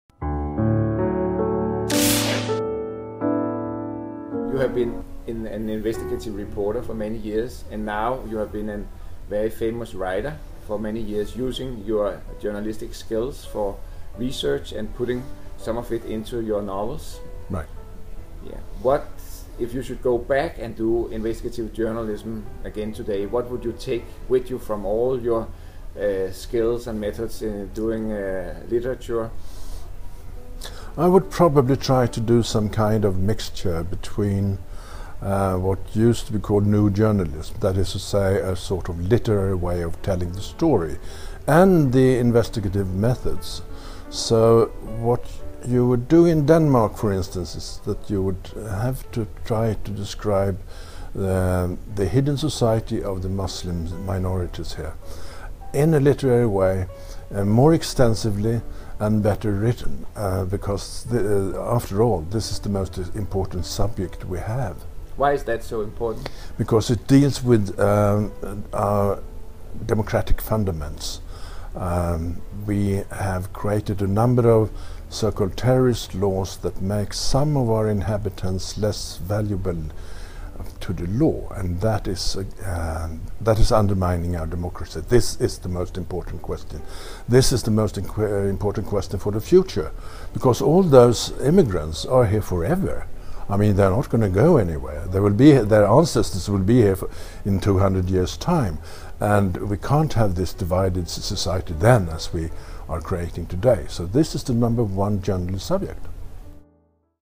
Jan Guillou - Investigative Reporting and the Most Important Subject
The famous writer Jan Guillou talks about how he would do investigative reporting using his knowledge from being an author. He suggests to focus on how muslims and minorities are living. This is the most important topic in a democratic world, says Guillou.